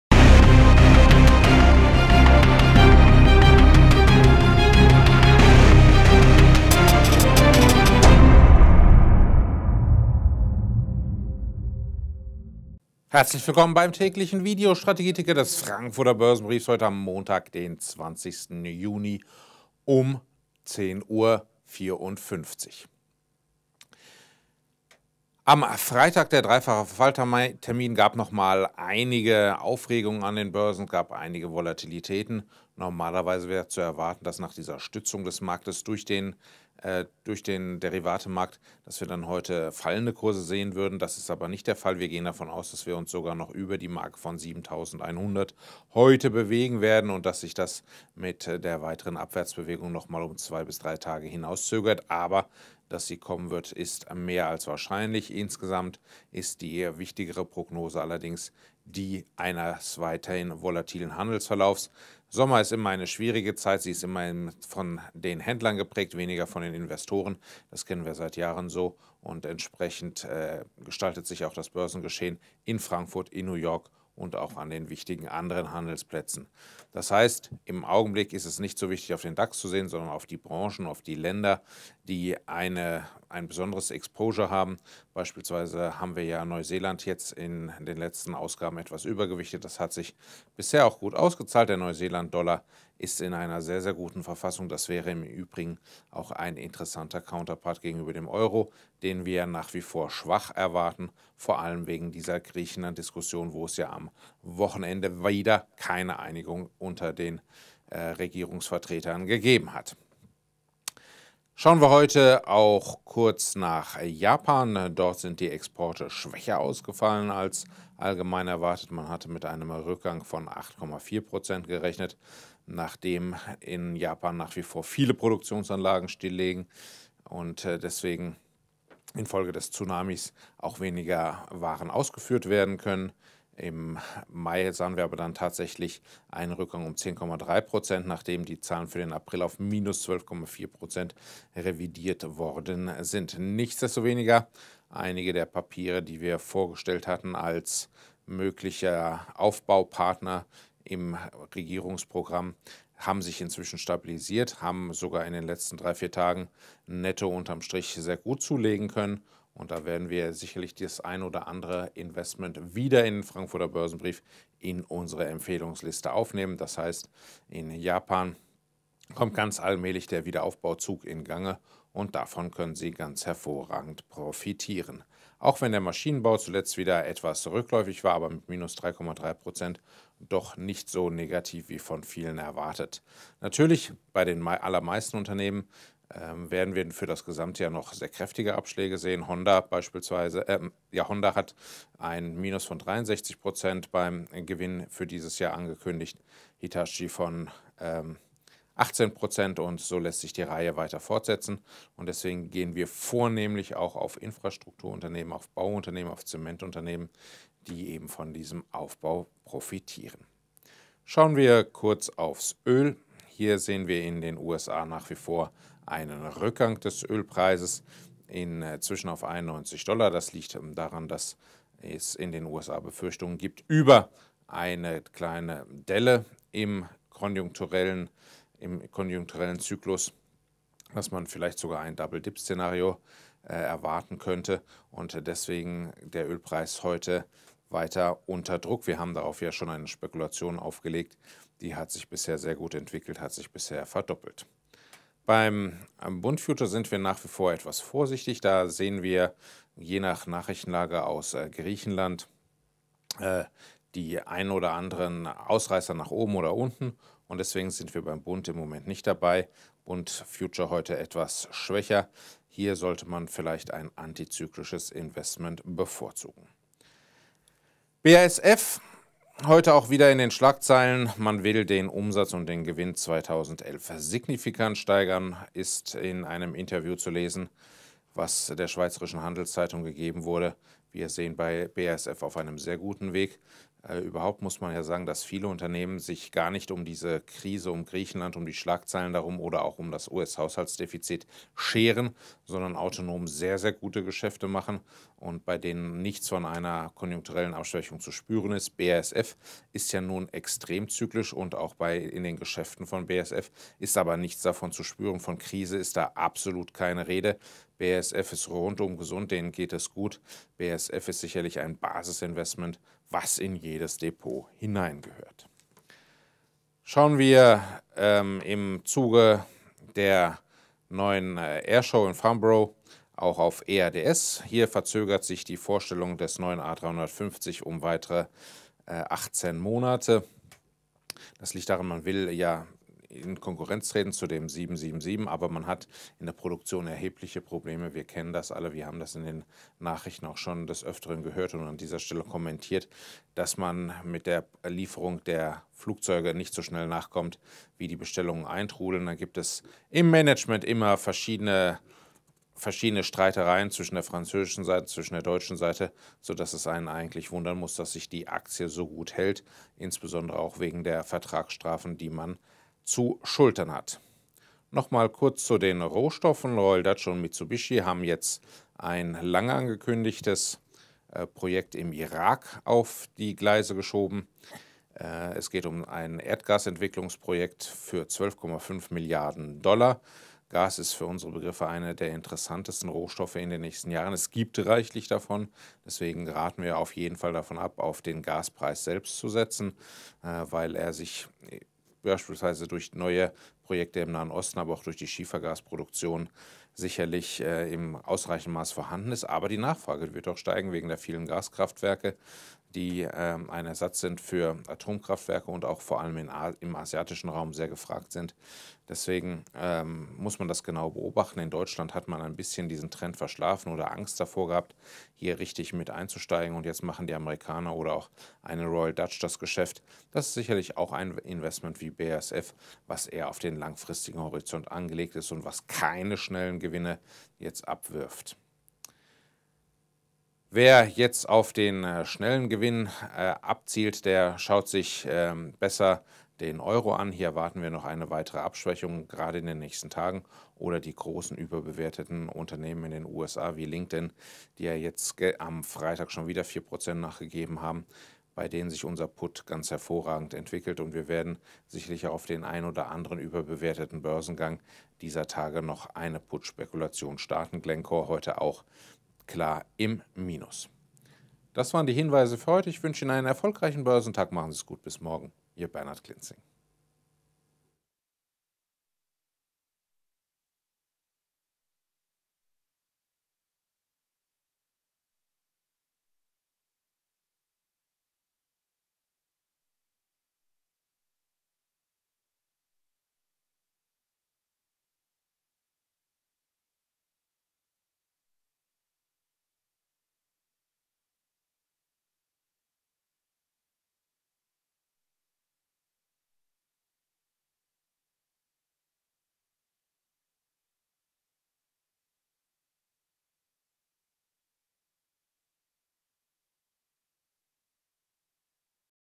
Börsenstrategie Ticker - Frankfurter Börsenbrief - 20.06.2011
Der Sommer wird schwierig!
Aktien: EADS, BASF & Royal Dutch Shell